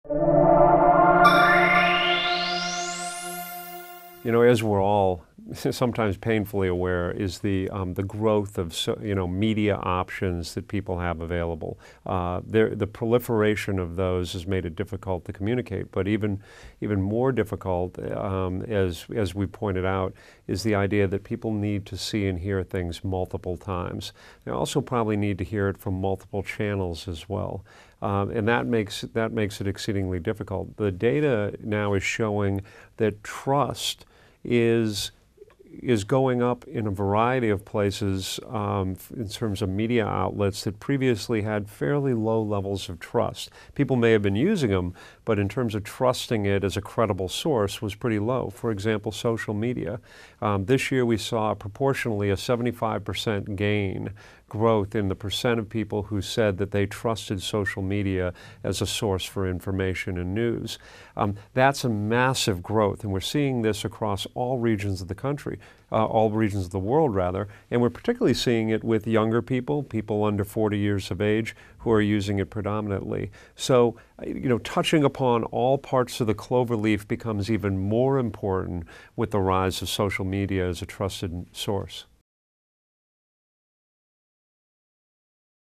Steve Lombardo on the proliferation of media channels
Steve Lombardo, CEO of Edelman's independent Research firm StrategyOne, discusses below how the proliferation of media is challenging.